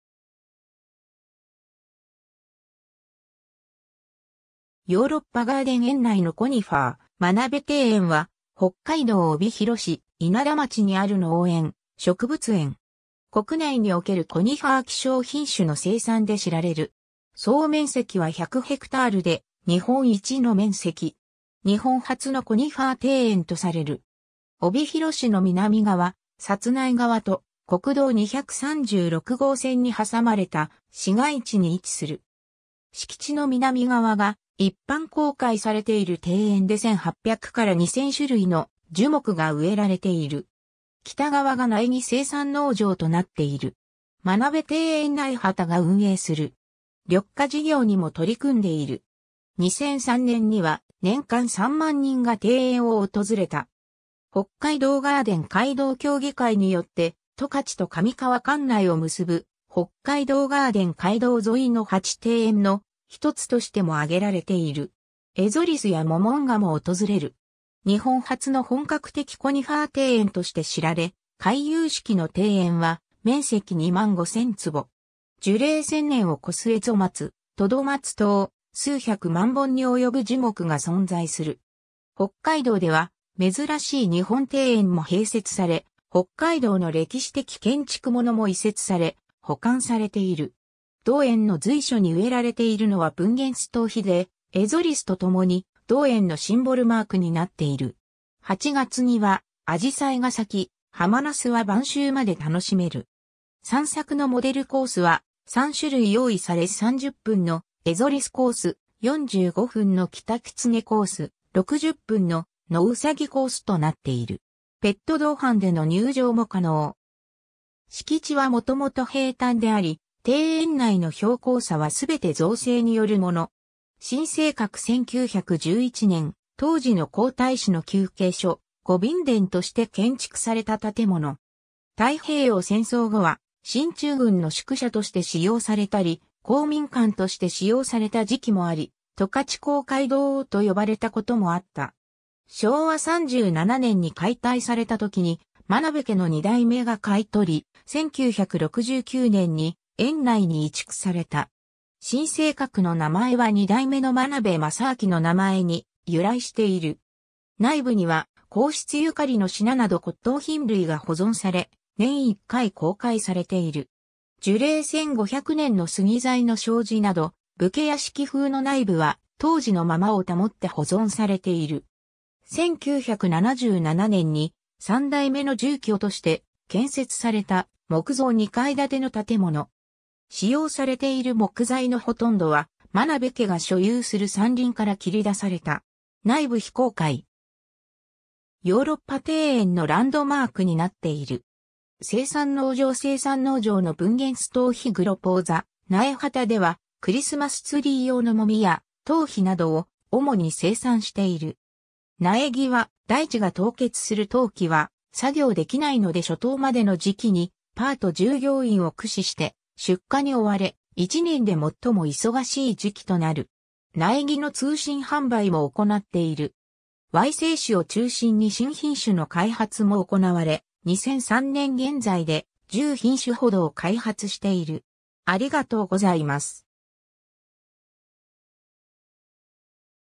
真鍋庭園
ヨーロッパガーデン 園内のコニファー 真鍋庭園（まなべていえん）は北海道帯広市稲田町にある農園、植物園。
国内におけるコニファー希少品種の生産で知られる。
総面積は100ヘクタールで日本一の面積。
日本初のコニファー庭園とされる。
 帯広市の南側、札内川と国道236号線に挟まれた市街地に位置する。
敷地の南側が一般公開されている庭園で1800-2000種類の樹木が植えられている。
北側が苗木生産農場となっている。
「真鍋庭園苗畑」が運営する。
緑化事業にも取り組んでいる。
2003年には年間3万人が庭園を訪れた。
北海道ガーデン街道協議会によって十勝と上川管内を結ぶ「北海道ガーデン街道」沿いの8庭園の一つとしても挙げられている。
エゾリスやモモンガも訪れる。
 日本初の本格的コニファー庭園として知られ、回遊式の庭園は面積25,000坪。
樹齢1000年を越すエゾ松、トド松等、数100万本に及ぶ樹木が存在する。
北海道では珍しい日本庭園も併設され、北海道の歴史的建築物も移設され保管されている。
同園の随所に植えられているのはプンゲンストウヒで、エゾリスとともに同園のシンボルマークになっている。
8月にはアジサイが咲き、ハマナスは晩秋まで楽しめる。
散策のモデルコースは、3種類用意され30分のエゾリスコース、45分のキタキツネコース、60分のノウサギコースとなっている。
ペット同伴での入場も可能。
敷地は元々平坦であり庭園内の標高差はすべて造成によるもの。
 真正閣(奥側) 1911年（明治44年）、当時の皇太子（大正天皇）の休憩所「御便殿」（3室）として建築された建物。
太平洋戦争後は進駐軍の宿舎として使用されたり、公民館として使用された時期もあり、「十勝公会堂」と呼ばれたこともあった。
昭和37年に解体されたときに眞鍋家の2代目が買い取り、1969年（昭和43年）に園内に移築された。
真正閣の名前は2代目の真鍋正明の名前に由来している。
内部には皇室ゆかりの品など骨董品類が保存され、年一回公開されている。
樹齢1500年の杉材の障子など、武家屋敷風の内部は当時のままを保って保存されている。
 1977年に、3代目の住居として建設された木造2階建ての建物。
使用されている木材のほとんどは真鍋家が所有する山林から切り出された。
内部非公開。
ヨーロッパ庭園のランドマークになっている。
 生産農場 生産農場のプンゲンストウヒ　グロポーザ 苗畑ではクリスマスツリー用のモミやトウヒなどを主に生産している。
苗木は大地が凍結する冬季は作業できないので初冬までの時期にパート従業員を駆使して出荷に追われ、1年で最も忙しい時期となる。
苗木の通信販売も行っている。
矮性種を中心に新品種の開発も行われ、2003年現在で10品種ほどを開発している。